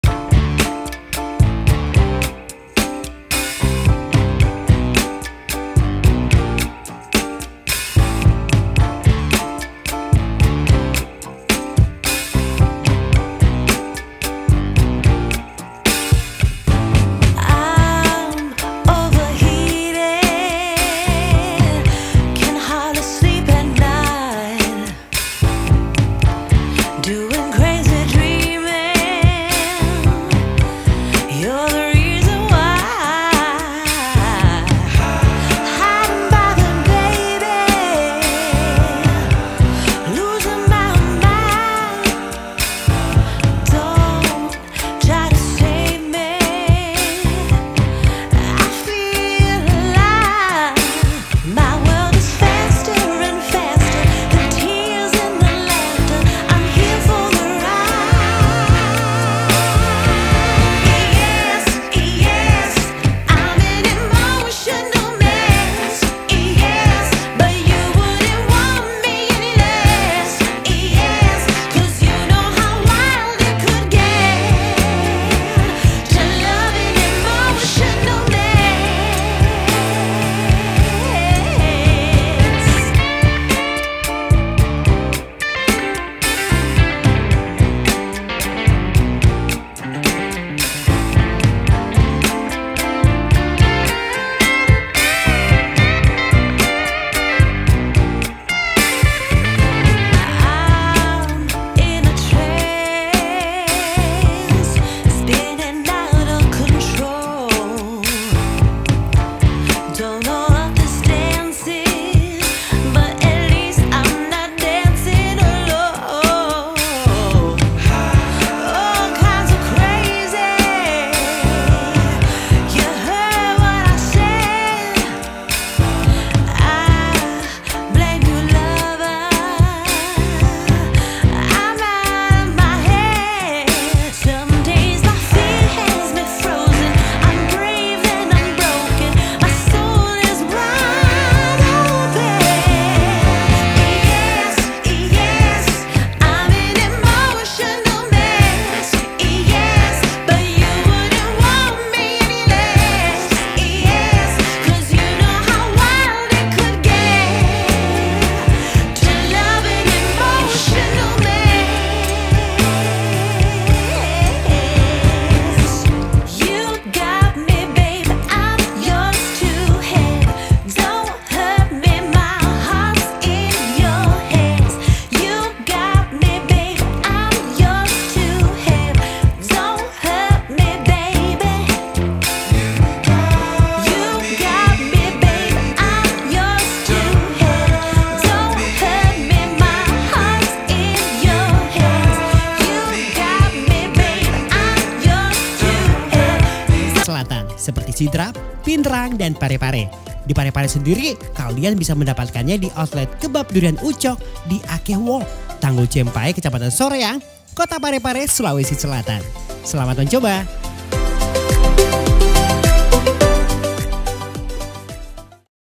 MENCOBA LEZATNYA DURIAN DALAM KEMASAN KEBAB UCOK DURIAN DENGAN VARIAN RASA BERAGAM
Halo penikmat camilan kekinian. salam jumpa.
kali ini saya perkenalkan kebab durian ucok. dengar namanya sudah bisa di tebak. ini adalah kebab. tapi bukan kebab sembarangan. Mari kita coba.

Sekarang kamu bisa mencoba kebab durian ucok ini di beberapa daerah di wilayah ajatappareng sulawesi selatan. Seperti sidrap pinrang dan parepare. Di Parepare sendiri kalian bisa mendapatkannya di outlet kebab durian ucok di akeh walk tanggul cempae kecamatan soreang kota Parepare sulawesi selatan.